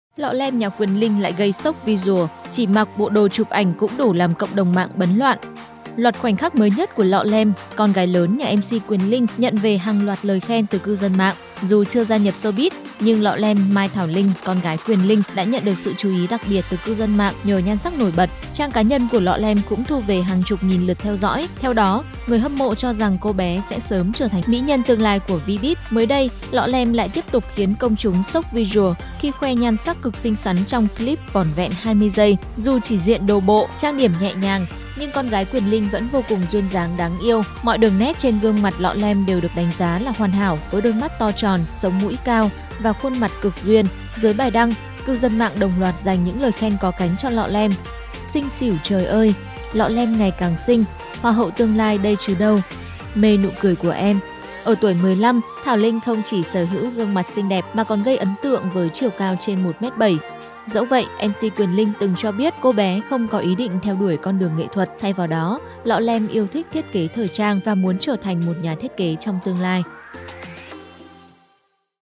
Lọ Lem nhà Quyền Linh gây “sốc visual”, chỉ mặc đồ bộ chụp ảnh cũng đủ làm CĐM bấn loạn | VIETDRAMA
Lọ Lem nhà Quyền Linh lại gây “sốc visual”, chỉ mặc đồ bộ chụp ảnh cũng đủ làm CĐM bấn loạn

Loạt khoảnh khắc mới nhất của Lọ Lem - con gái lớn nhà MC Quyền Linh nhận về hàng loạt lời khen từ cư dân mạng.
Nội dung: Ngọc Anh (tổng hợp)